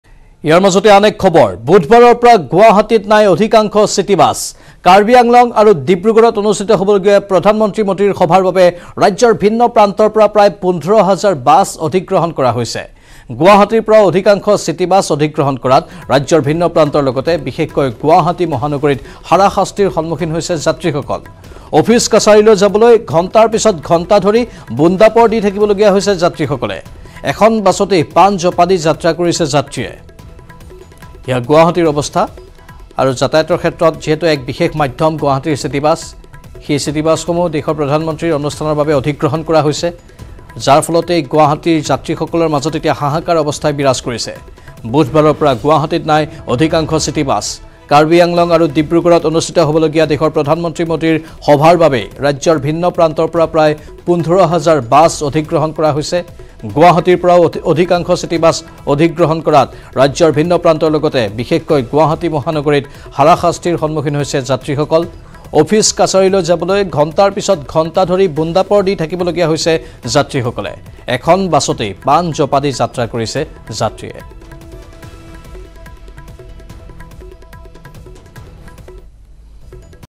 Guwahati City Bus | বুধবাৰৰ পৰা নাই গুৱাহাটীত চিটী বাছ
28 April, 2022 :  ৰাজ্যৰ বিভিন্ন প্ৰান্তৰ পৰা প্ৰায় ১৫হাজাৰ বাছ অধিগ্ৰহণ কৰা হৈছে । অফিছ কাছাৰীলৈ যাবলৈ ওলাই অহা মহানগৰবাসীৰ এতিয়া হাহাকাৰ। বাছ আস্থানতে বোন্দাপৰ দি থাকিবলগীয়া হৈছে যাত্ৰী ।